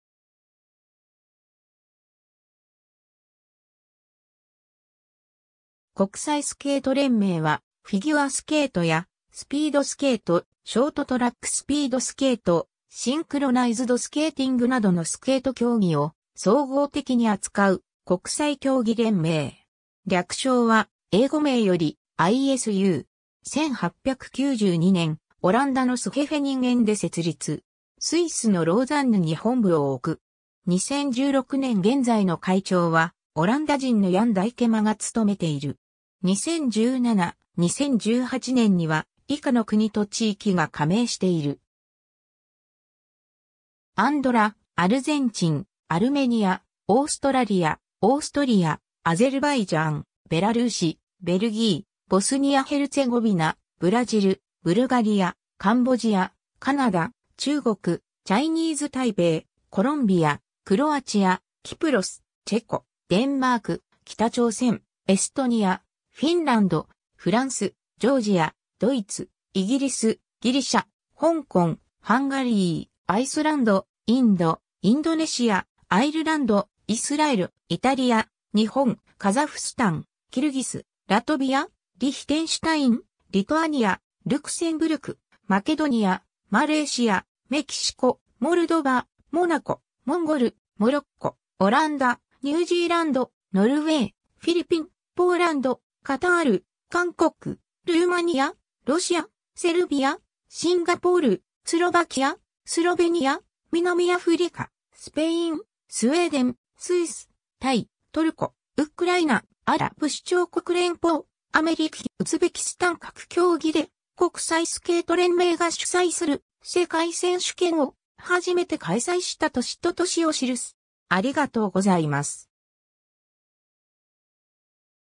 国際スケート連盟
国際スケート連盟（こくさいスケートれんめい）は、フィギュアスケートやスピードスケート、ショートトラックスピードスケート、シンクロナイズドスケーティングなどのスケート競技を総合的に扱う国際競技連盟。
略称は英語名よりISU。
 1892年、オランダのスヘフェニンゲンで設立。
スイスのローザンヌに本部を置く。
2016年現在の会長はオランダ人のヤン・ダイケマが務めている。
 2017/2018年には以下の国と地域が加盟している。